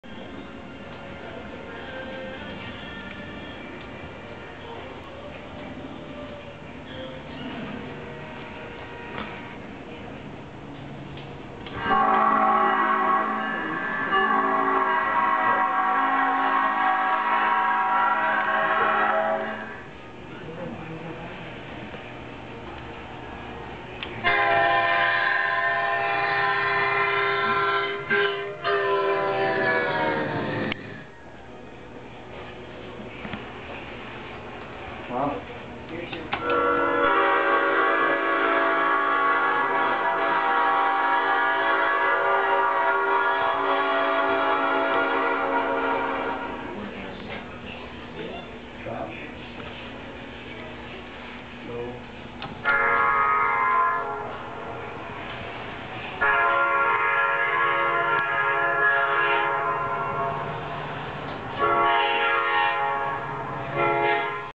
The Sound of Amtrak
From the 40th Anniversary Amtrak exhibit train, a variety of train whistles through the years.